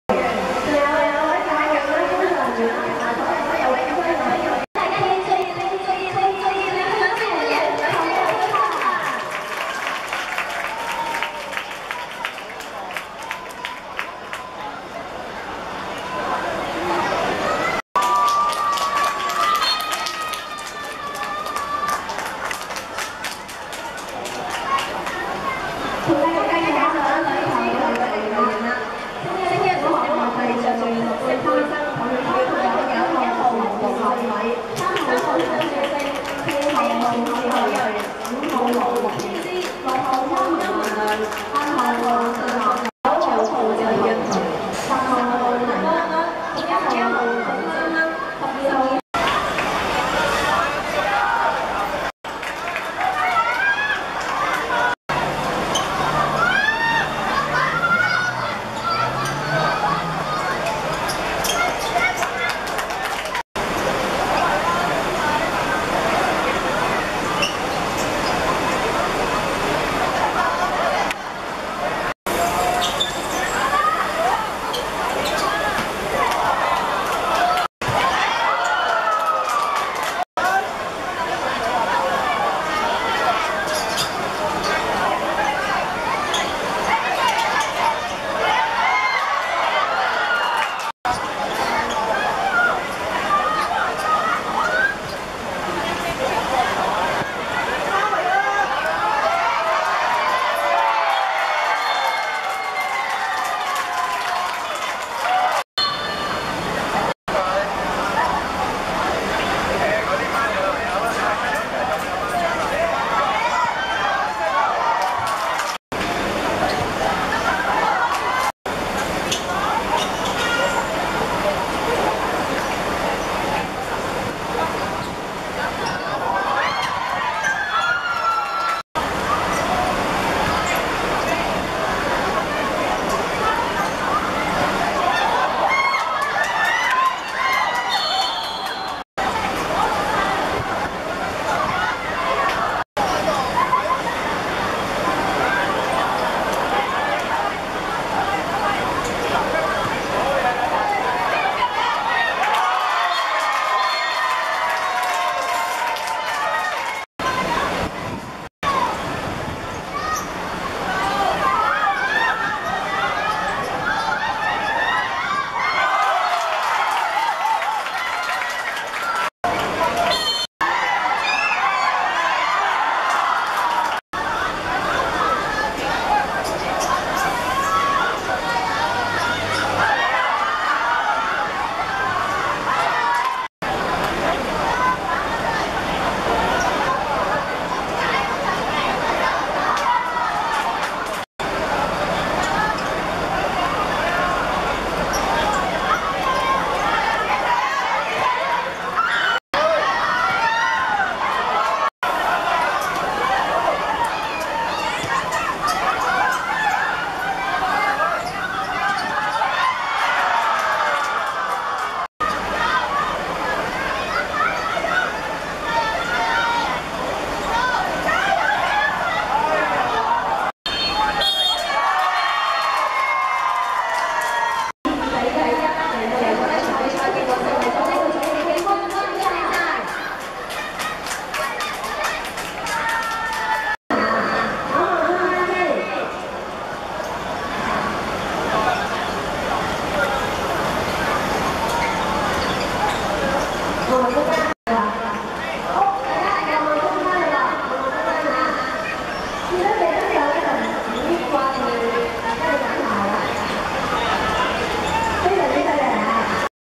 聖若瑟vs馬循(2014.7.5.小學五人足球精英賽~男子U13決賽)精華
2014.7.5.旺角麥花臣球塲 15:35 聖若瑟(綠) 2:1 馬循(紫藍)